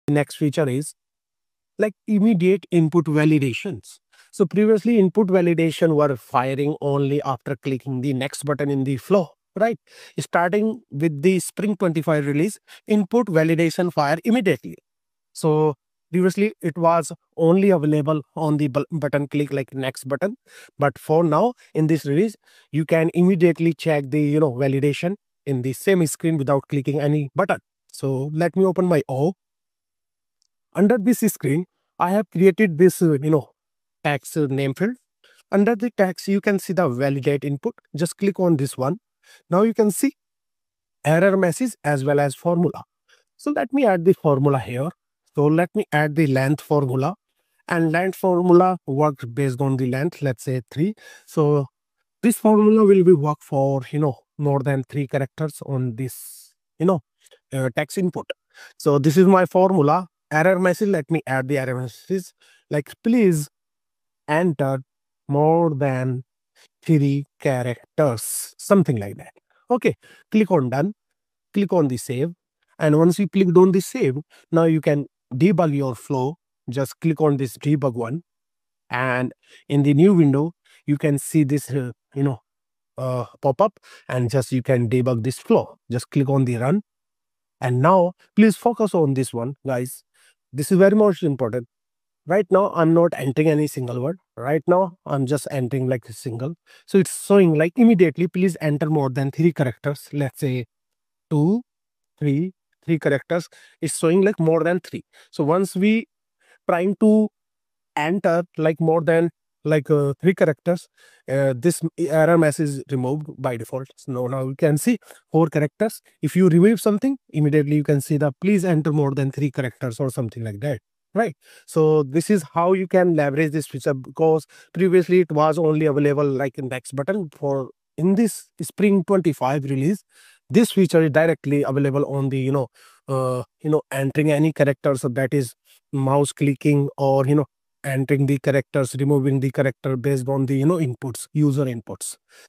🔴Spring '25! Salesforce Flow Instant Input Validation | @SalesforceHunt
Hello Everyone,
Discover the game-changing feature in Salesforce Spring '25: Instant Input Validation for Flows! 🚀 Enhance user experience and boost efficiency by validating input fields in real-time within your Flow screens.